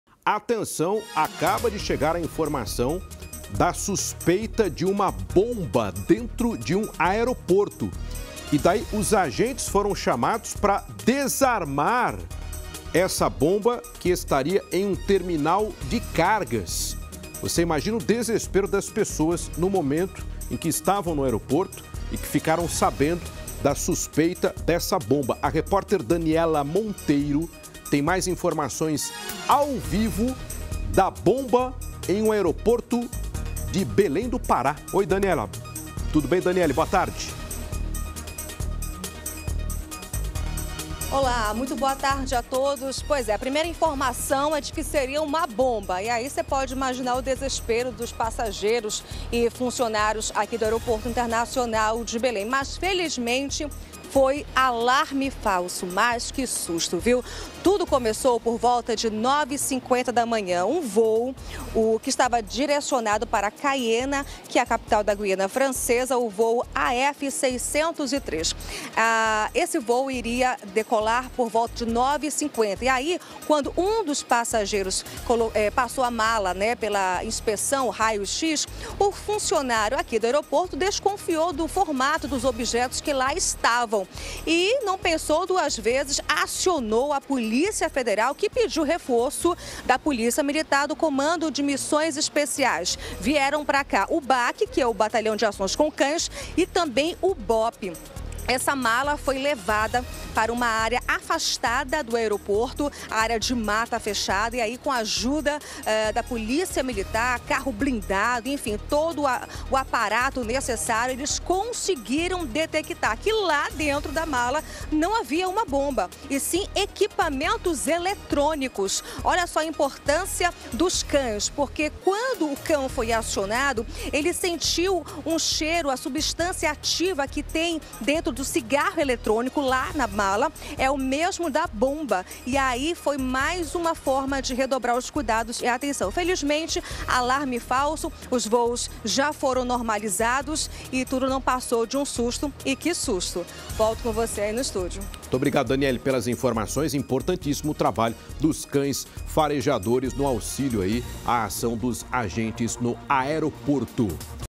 Suspeita de bomba deixa passageiros de aeroporto de Belém em alerta
Uma suspeita de bomba foi reportada no aeroporto internacional de Belém do Pará neste sábado (18). O alerta ocorreu durante a inspeção de bagagens para um voo com destino à Guiana Francesa, na manhã de hoje (18). As autoridades foram acionadas e equipes especializadas foram mobilizadas para verificar o objeto suspeito dentro de uma mala. Após investigação minuciosa, constatou-se que se tratava apenas de equipamentos eletrônicos e não havia ameaça real. Os voos foram normalizados e a situação foi controlada sem incidentes graves.